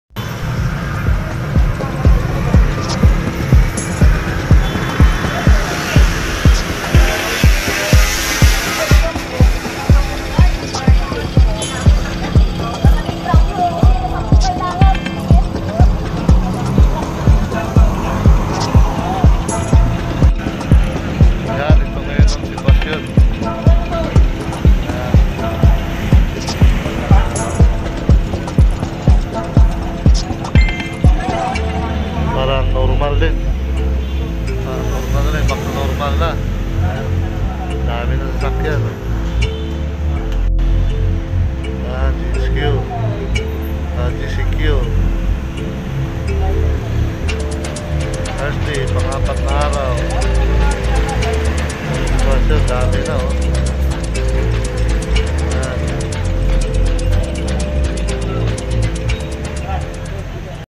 Day 4 in GCQ
Video recording at Pag-ibig fund in Shaw boulevard situation during my personal transaction in the office.